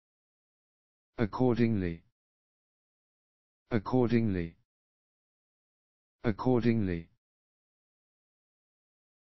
How to Pronounce accordingly
How to Pronounce accordingly with UK
English Phonetic UK [əˈkɔː(r)dɪŋli]